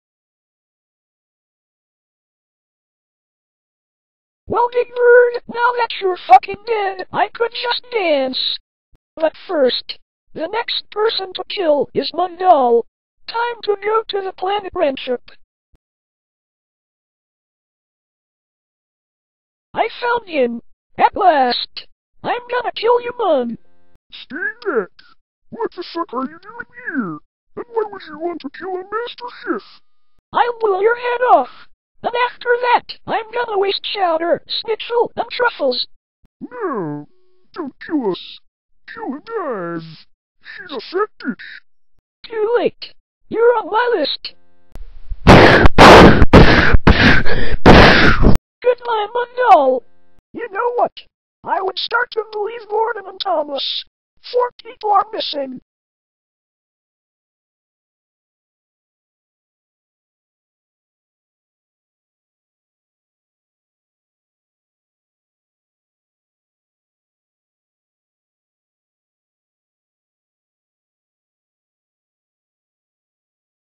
Elmo Kills Mung Daal
Elmo strikes again. Can this furry red murderer be stopped!